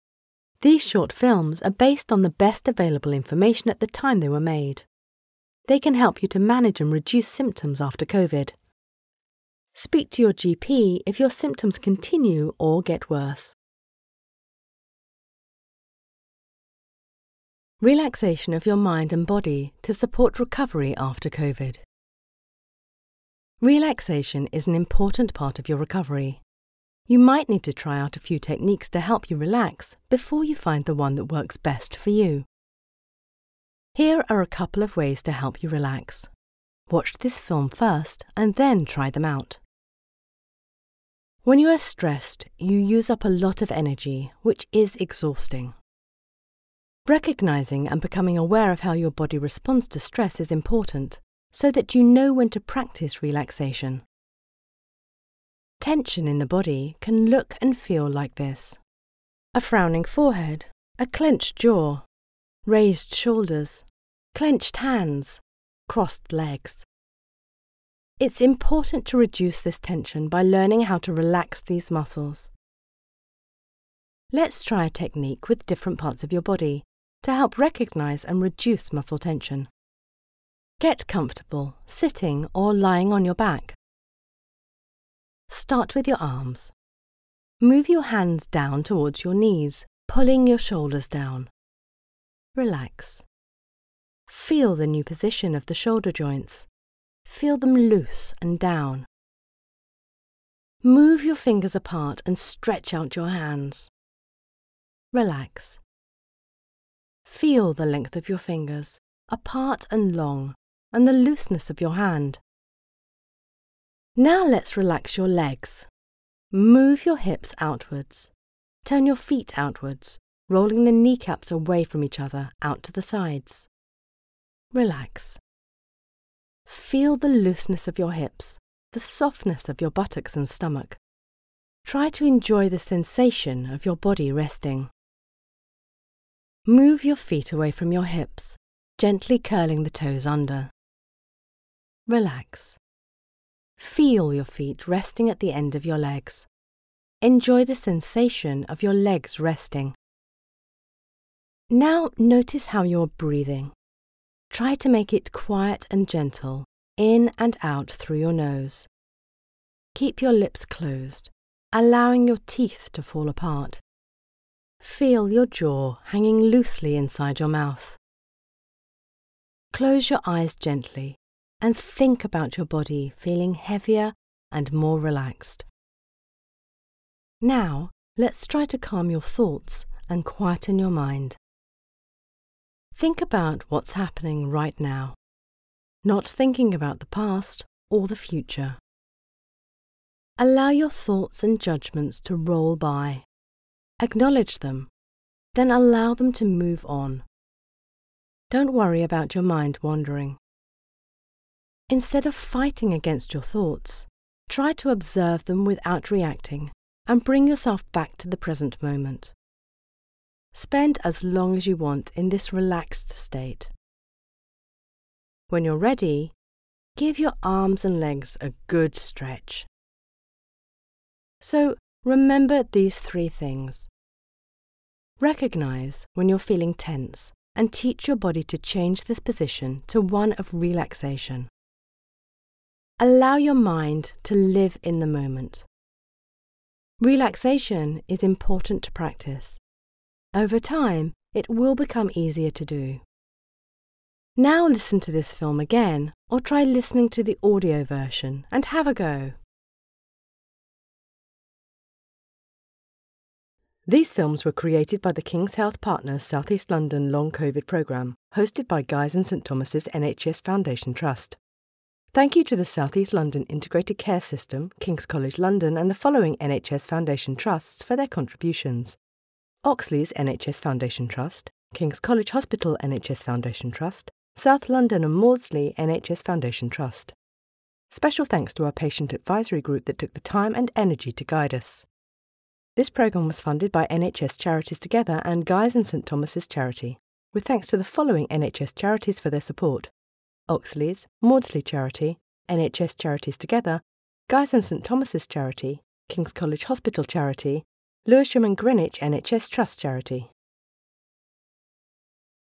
Relaxation of your mind and body: COVID recovery
If you’re feeling anxious or stressed, this film has advice to help you rest and relax properly. By giving yourself time, your mind and body have the chance to recover from the emotional effects of illness.

Find information and guidance to help your recovery from COVID on our

We'd like to know what you think of our films, and it can help us to improve our information for patients.